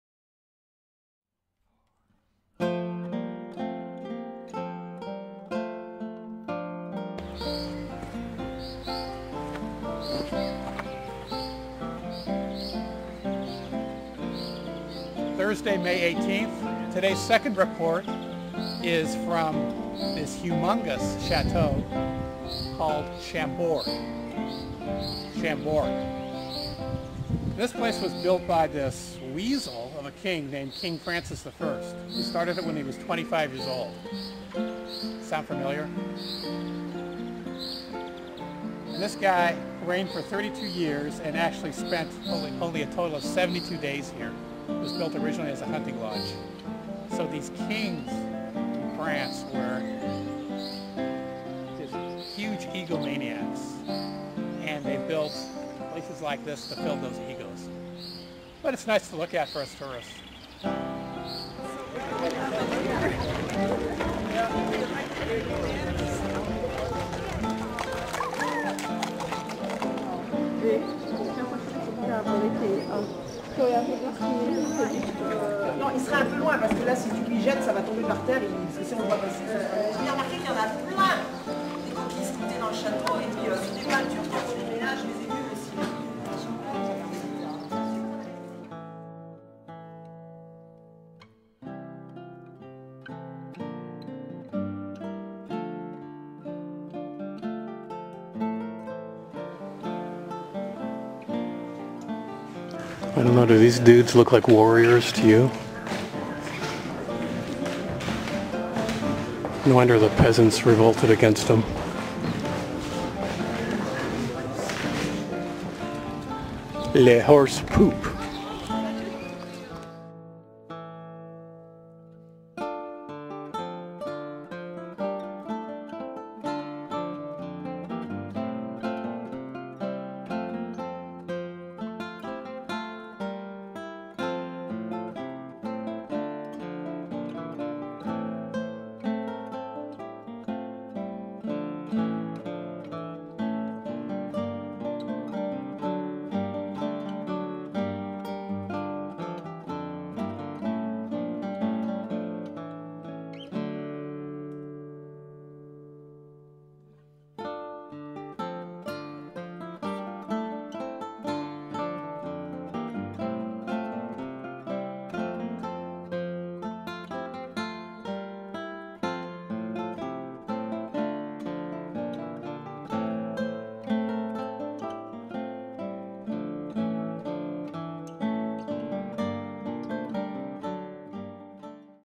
Loire Valley Part 2
A visit to Chambord chateau and Chartres, France.